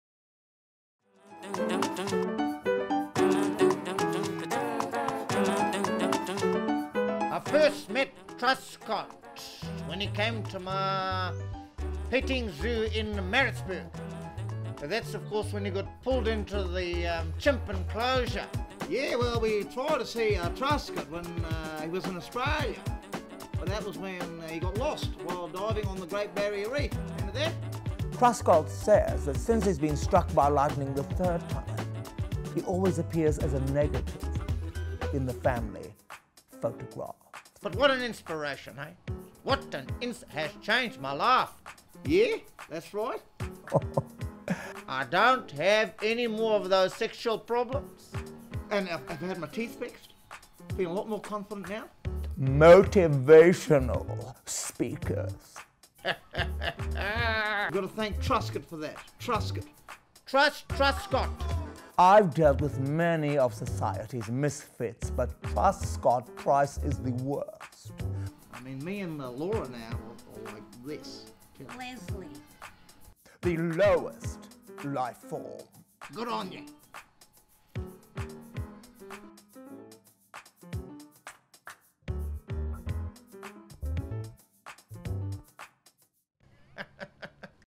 The Loser
The Loser's back! Due to insatiable public demand, the highly acclaimed and hilarious "motivational speaking" musical comedy show, The Loser, starring Durban's multi-award-winning comedian Aaron McIlroy in one of his best solo performances, returns to the Elizabeth Sneddon Theatre from 11 to 23 September 2012.